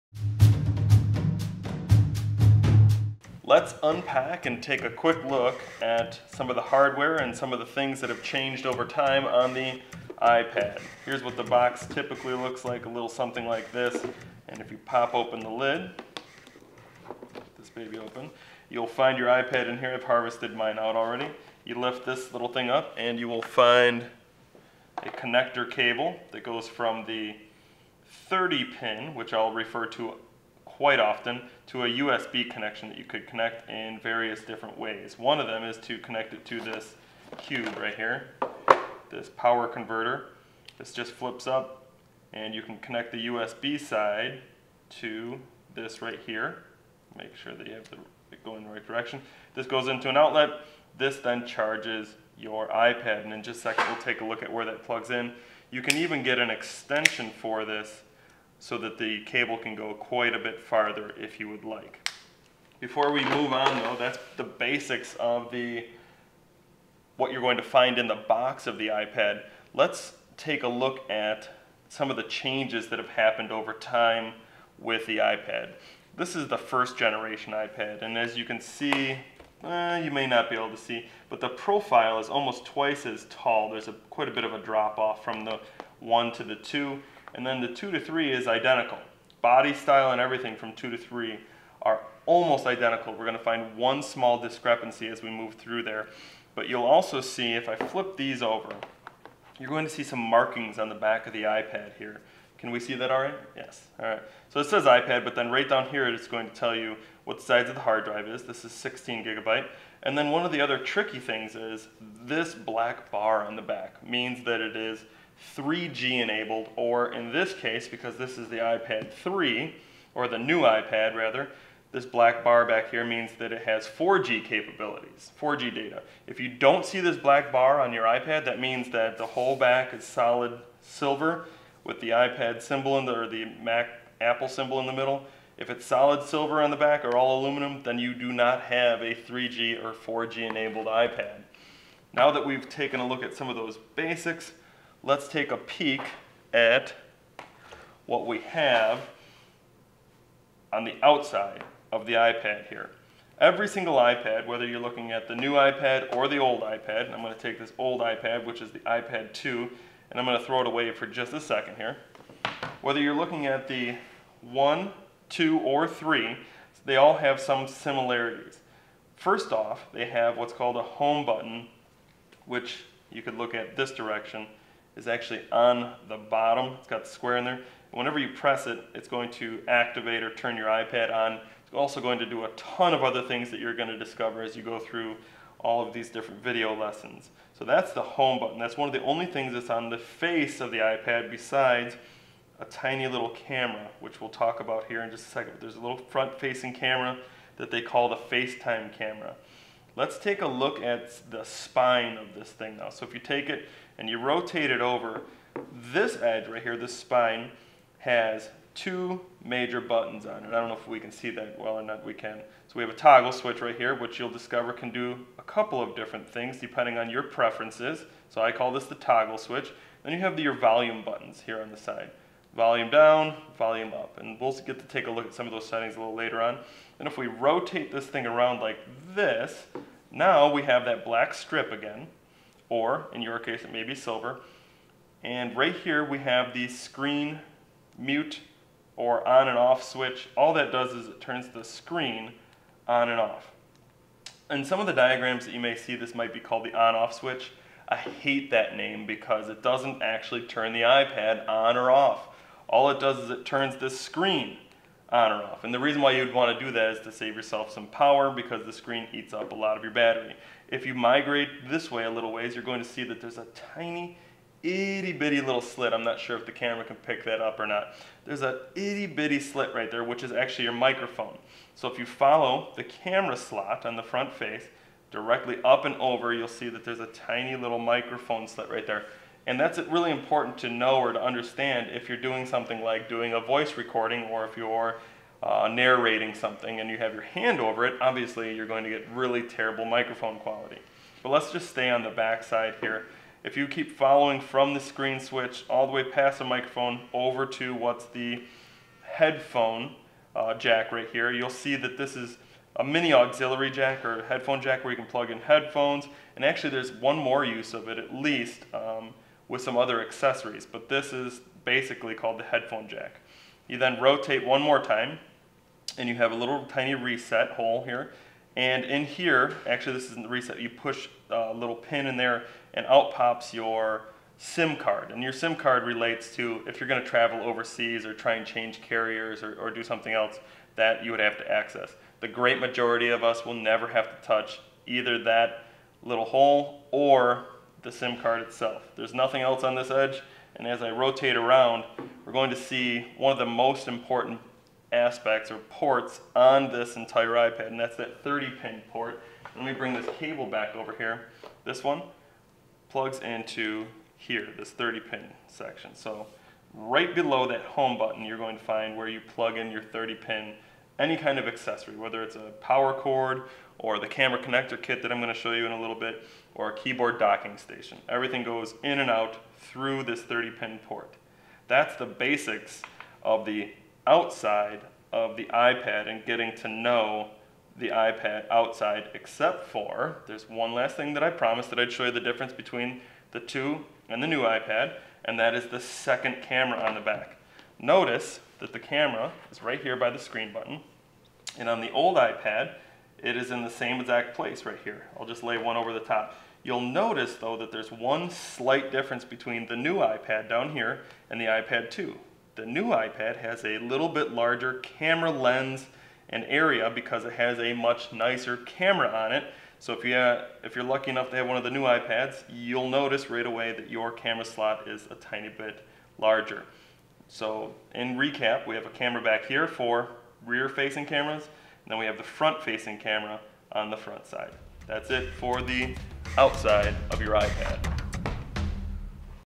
iPad- LECTURE- Out Of the Box
This video walks you through the first few steps on your road to successful iPad use. Join us as we look through a few of the essential characteristics of an iPad, and preview some of the tangible and technological uses of this device.